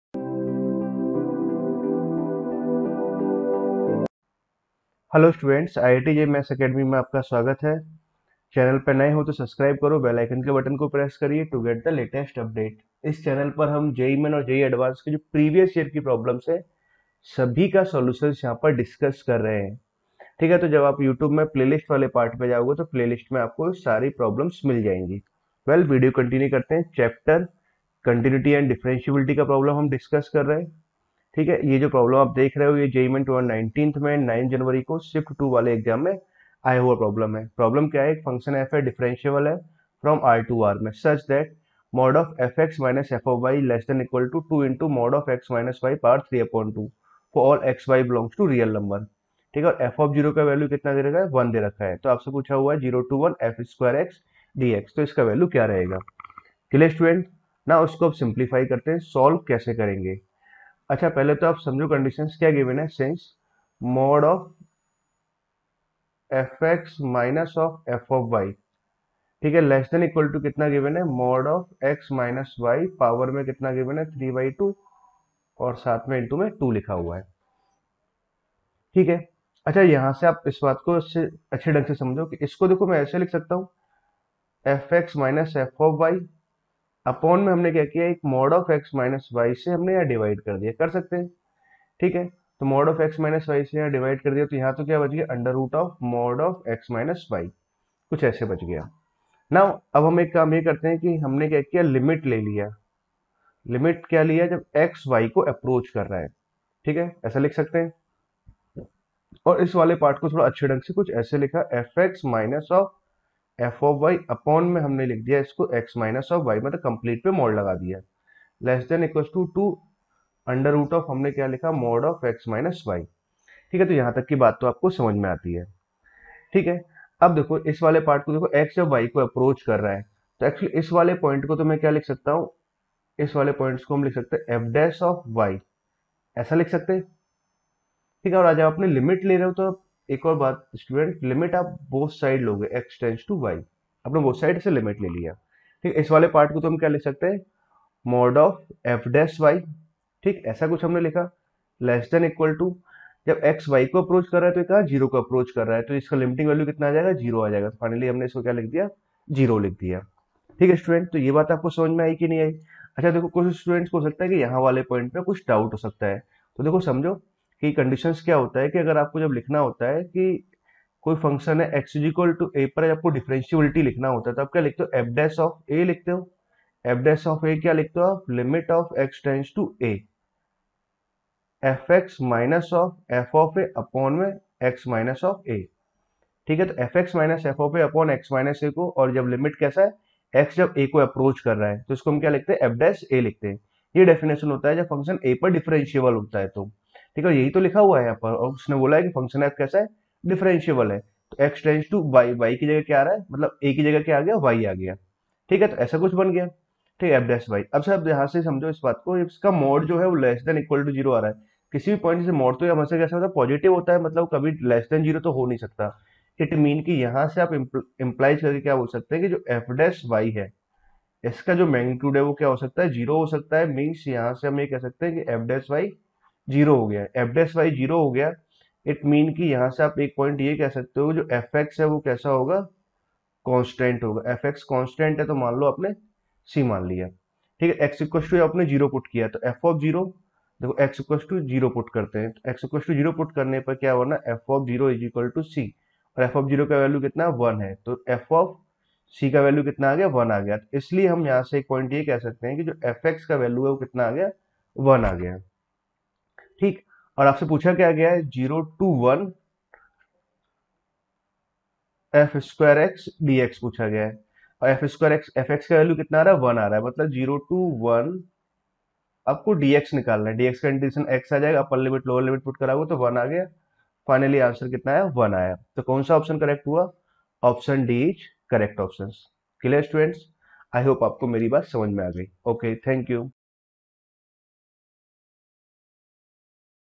continuity differentiability jee main 2019 9 january shift 2 by rs sir
IIT JEE MATHS ACADEMY
BY RAJENDRA SIR

JEE MAIN PREVIOUS YEAR VIDEO SOLUTION CHAPTER WISE
CHAPTER : continuity and differentiability